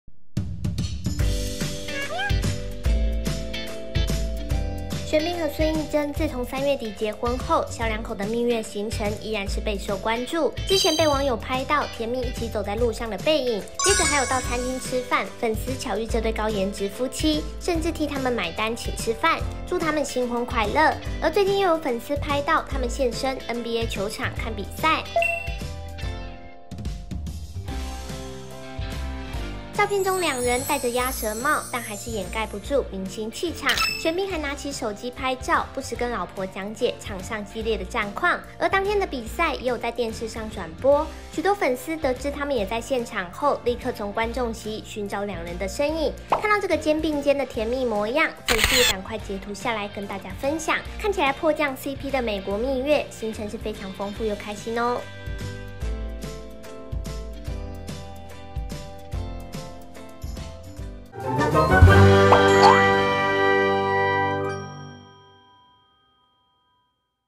玄彬孫藝真又被拍到在這裡出現!! 粉絲從電視看到狂截圖「好甜喔」
玄彬和孫藝真自從3月底結婚後
小倆口的蜜月行程依然是備受關注
而最近又有粉絲拍到
他們現身NBA球場看比賽
照片中兩人戴著鴨舌帽
但還是掩蓋不住明星氣場